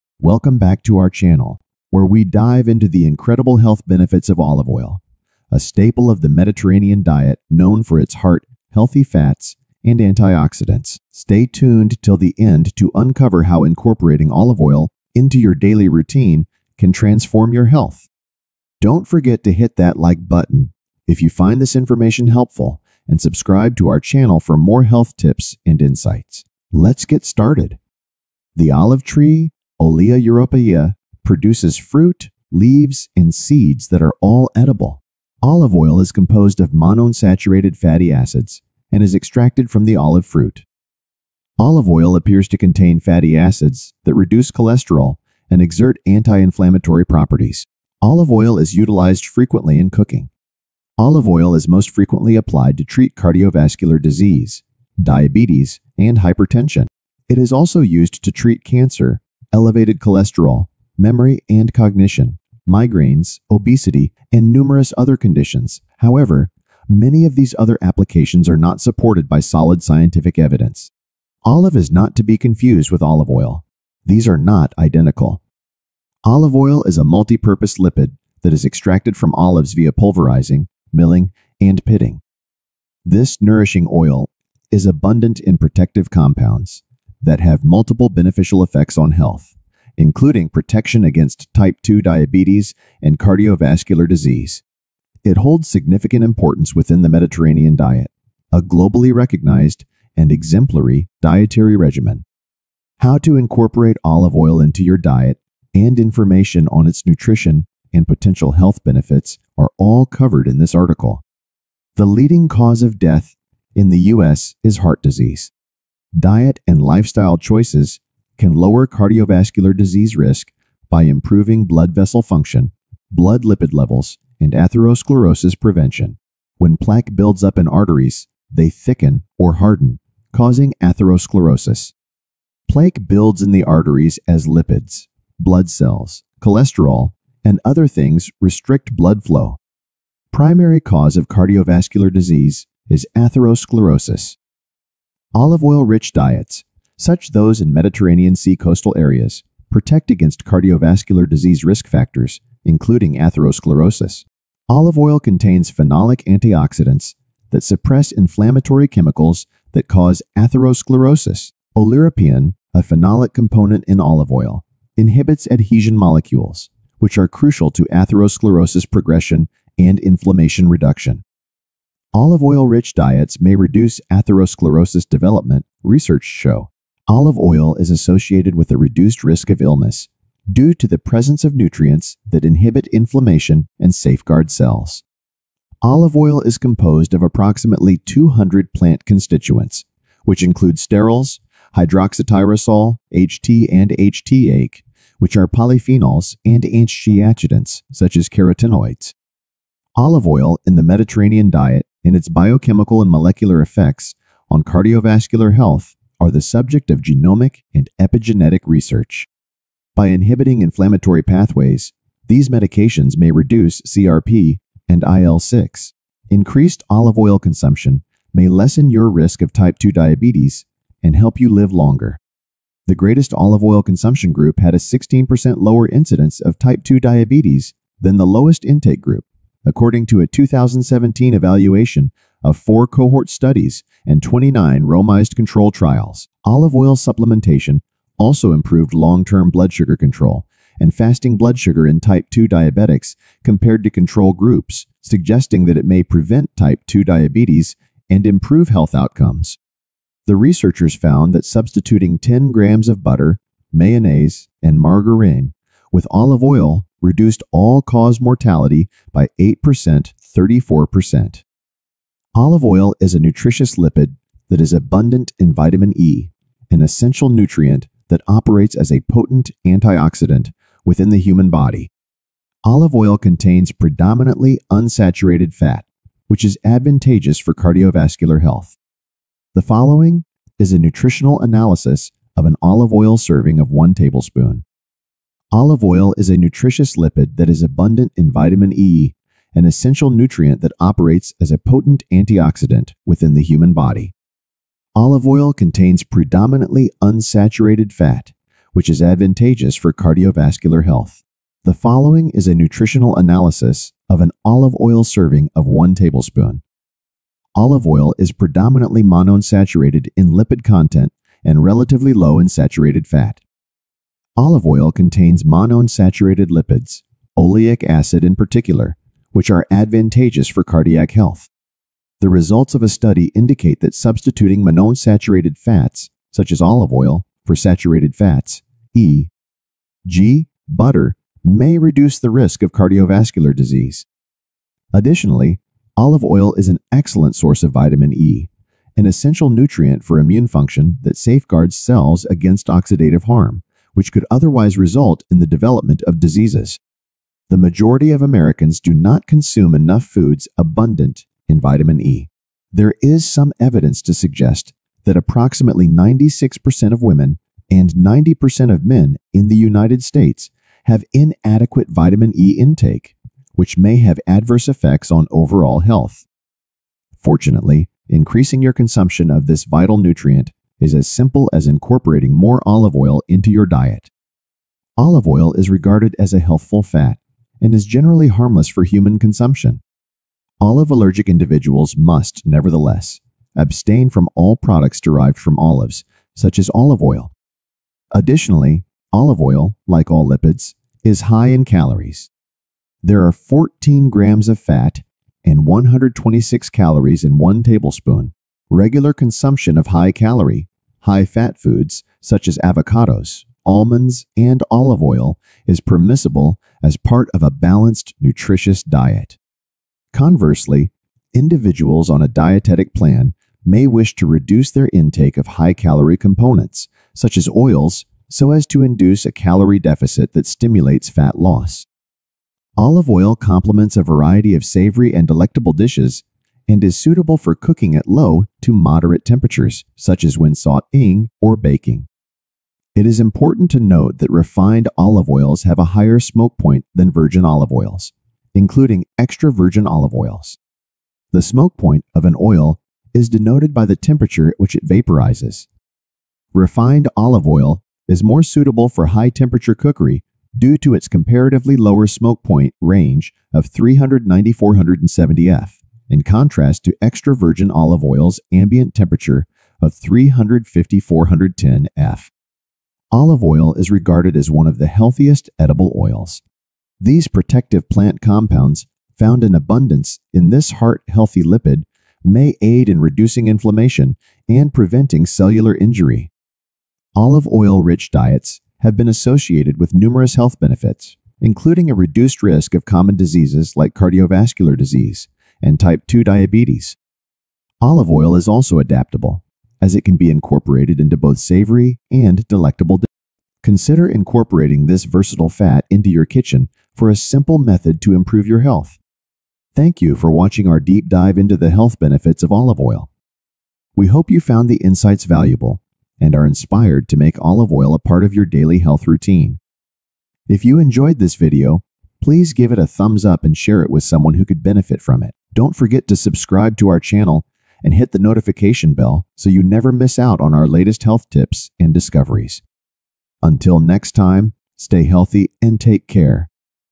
Olive Oil benefits | Benefits of Olive oil | Olive oil for hair
In this eye-opening video, we delve into the remarkable world of olive oil and its astounding health benefits. From heart health to glowing skin, olive oil has been cherished for centuries for its numerous medicinal properties. Join us as we uncover the science behind this liquid gold, exploring its role in reducing inflammation, promoting brain health, and even aiding in weight management. Whether you're a culinary enthusiast or simply looking to enhance your well-being, this video is packed with valuable insights and practical tips on incorporating olive oil into your daily routine. Don't miss out on the opportunity to revolutionize your health with nature's elixir – olive oil!